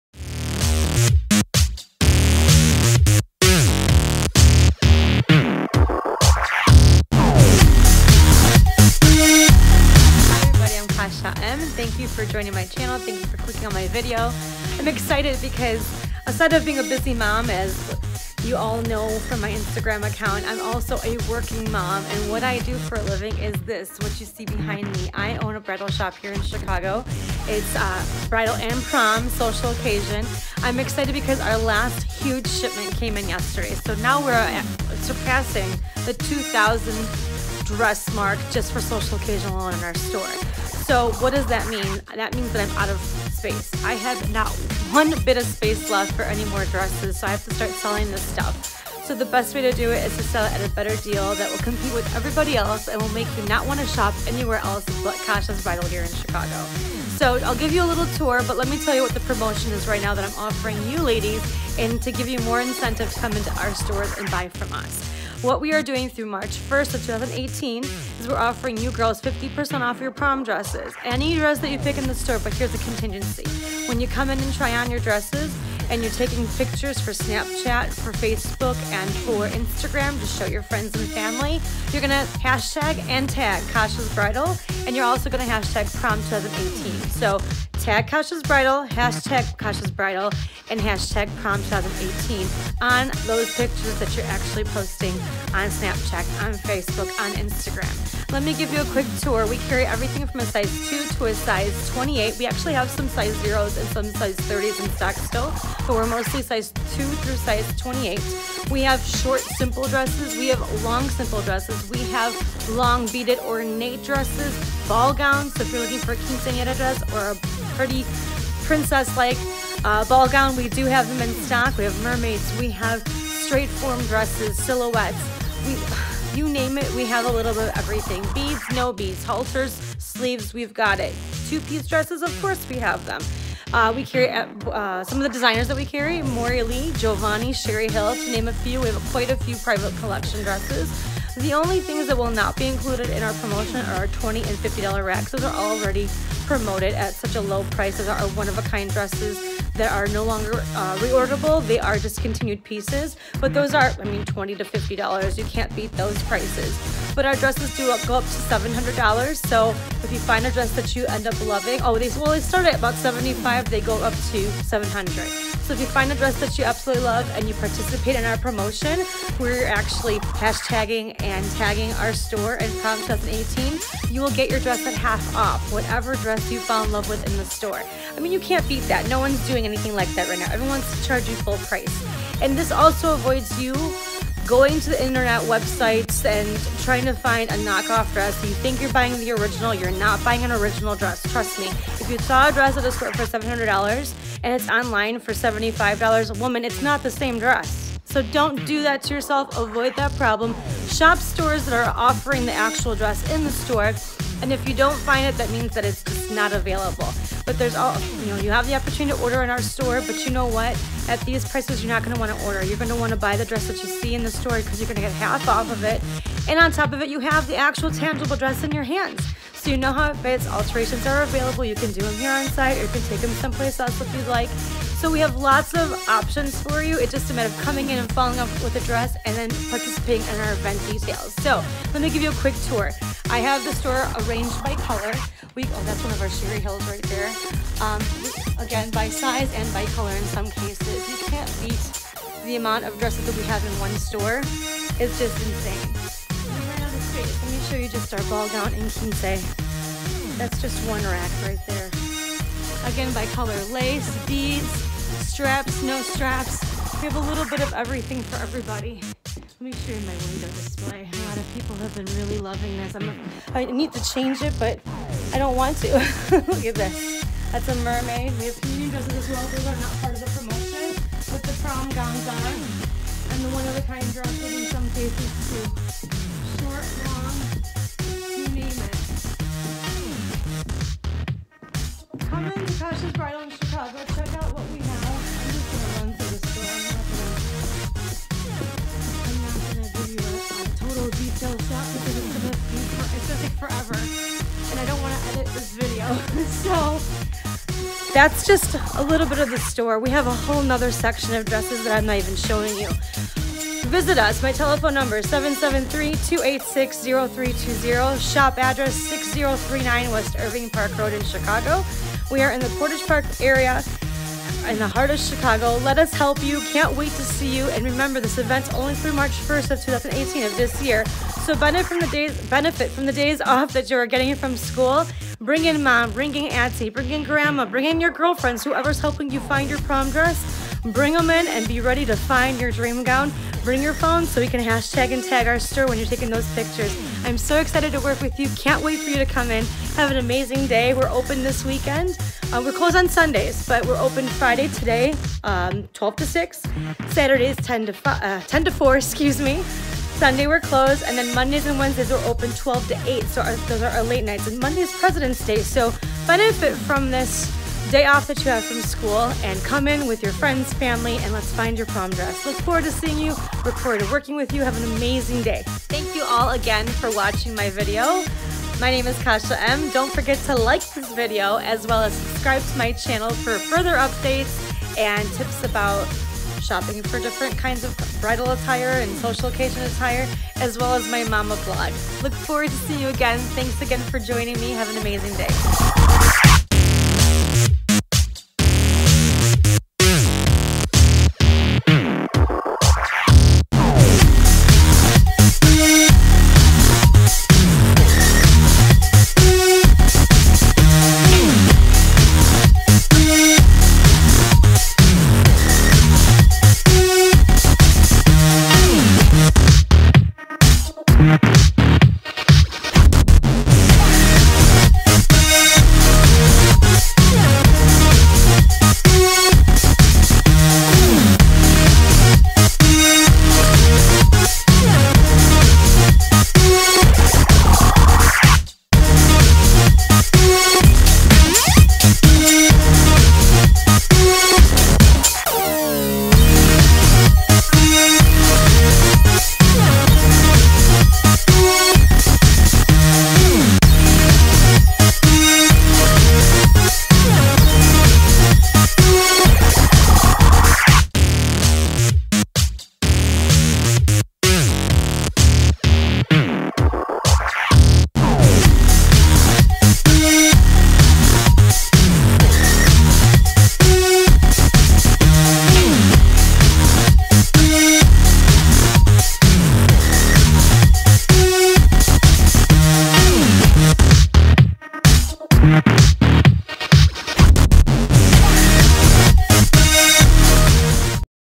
Kasia's Bridal, Prom Promo 2018
Giving you a tour of a portion of my shop (Kasia's Bridal & Special Occasion Boutique), and some insight as to what I do for a living. You won't believe how easy it is and it doesn't cost you a thing.